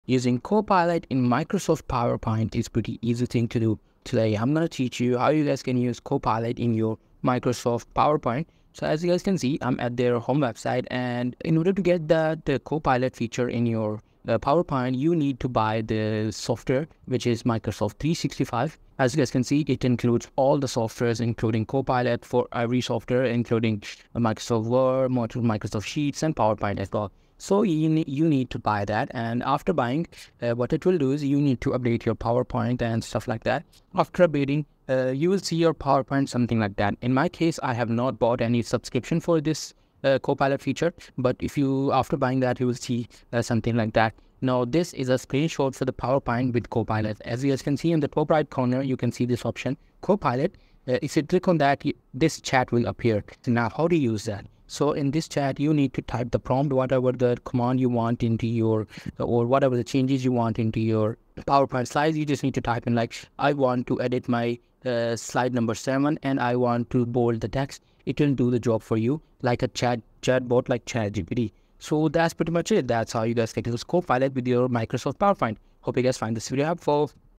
HOW TO USE MICROSOFT COPILOT IN POWERPOINT 2025! (FULL GUIDE)
Welcome to our comprehensive guide on "How To Use Microsoft Copilot In PowerPoint." In this video, we'll delve into the incredible capabilities of Microsoft Copilot and show you how to leverage it effectively in your PowerPoint presentations.

0:00 INTRO
0:04 TUTORIAL